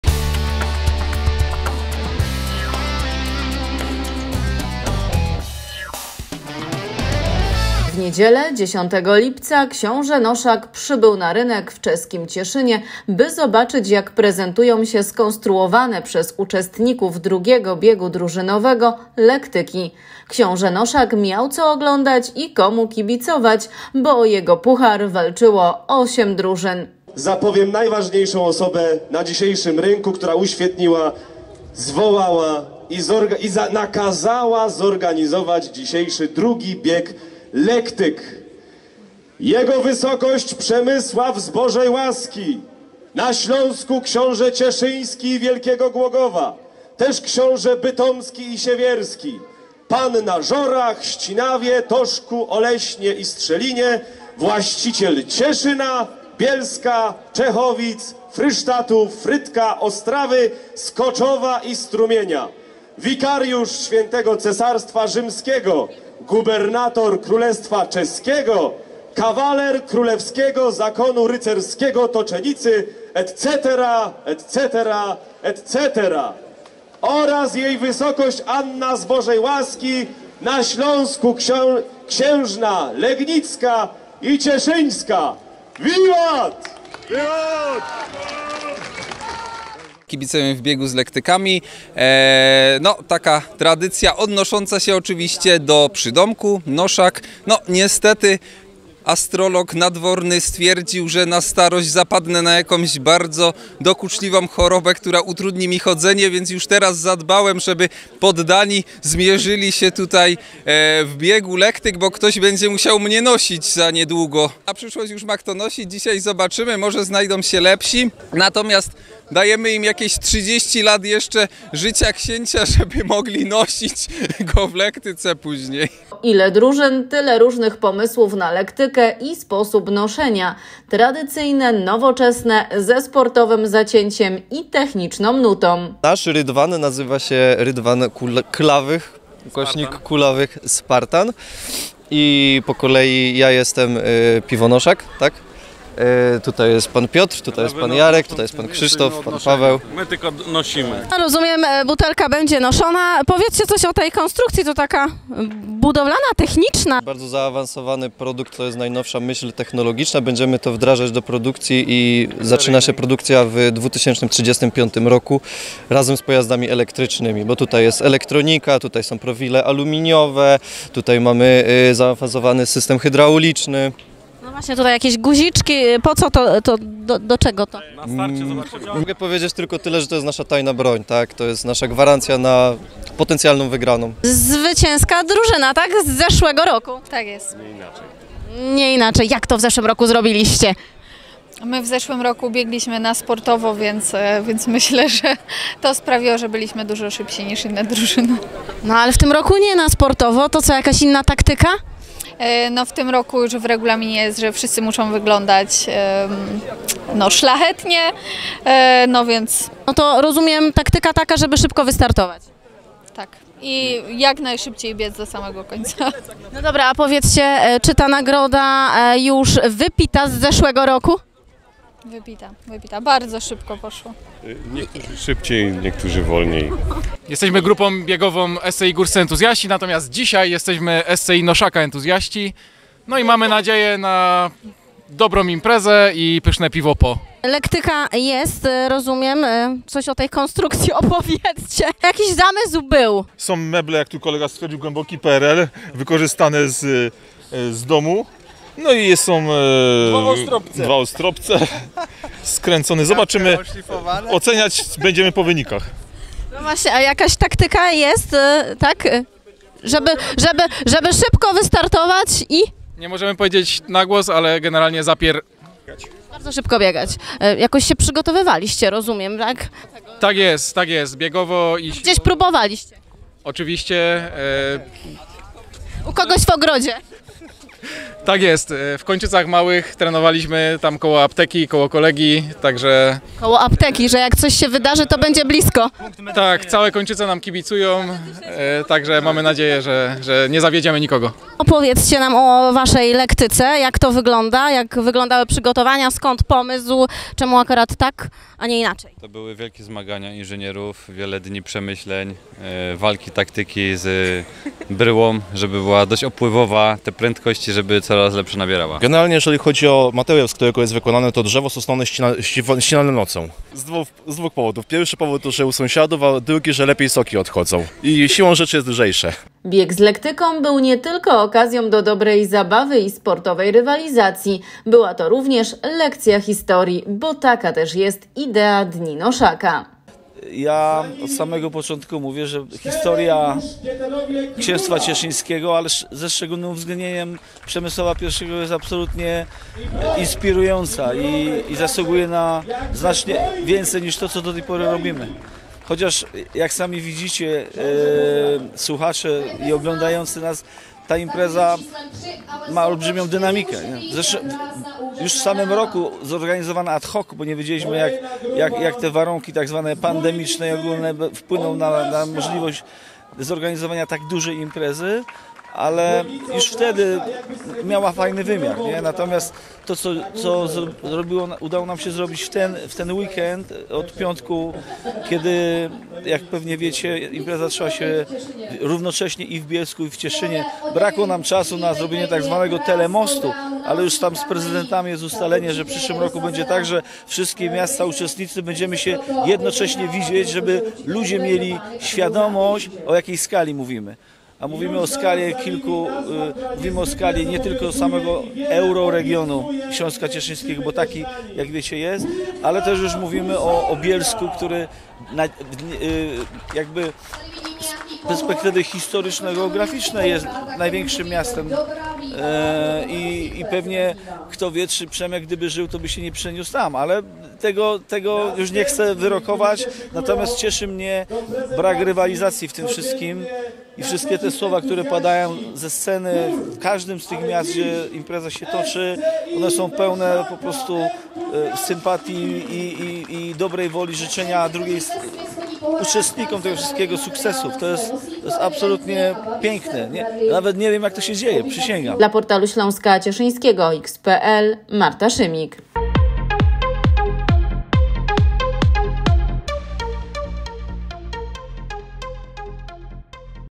Biegali z lektykami w Czeskim Cieszynie
W niedzielę (10.07) na rynku w Czeskim Cieszynie w ramach Dni Noszaka odbył się II Bieg Drużynowy z lektyką o Puchar Noszaka. Wystartowało 8 drużyn. Szlachetni Browarnicy powtórzyli ubiegłoroczny sukces i w tym roku również okazali się najszybsi. Drugie miejsce zajęła drużyna Zbójnicy Ondraszka, zaś trzecie 4 Muszkieterowie Króla. Tytuł najlepszej lektyki trafił do drużyny Piastowskie Orły.